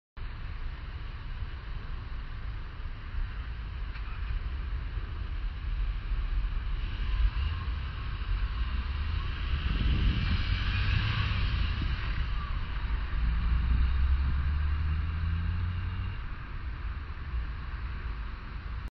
Über Rot fahren? Aber klar! Teil 2
Autofahrer (roter Kleinwagen) schläft und merkt Gelb zu spät (Gelbphase 2s), gibt trotzdem gas und fährt ziemlich genau 1s über Rot.

Amtliches Kennzeichen: FO-BL 2010

Der blaue Seat rechts hat Gelb auch missverstanden, kommt aber noch knapp vor Rot (~400ms) rüber.

Da soll mir einer sagen, Autofahrer würden sich an Regeln halten ^^